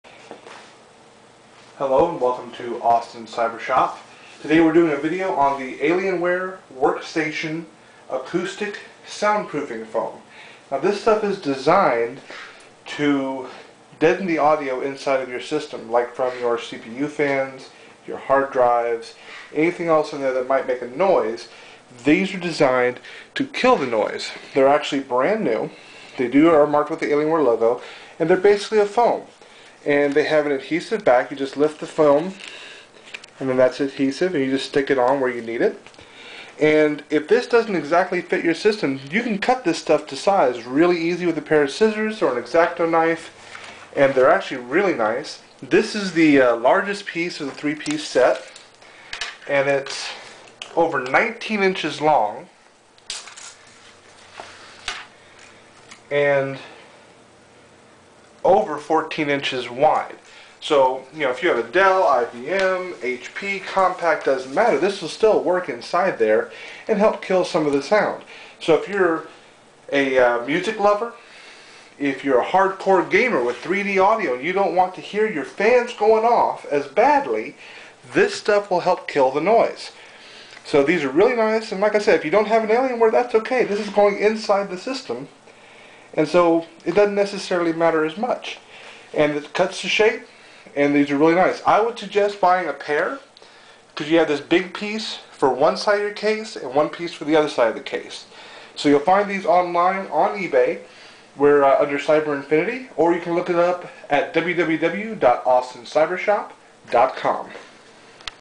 Alienware Acoustic SoundProofing Foam at Austin Cyber Shop
This is a video showing the Alienware Acoustic SoundProofing Foam we have for sale. This stuff is great for lowering the noise a computer makes.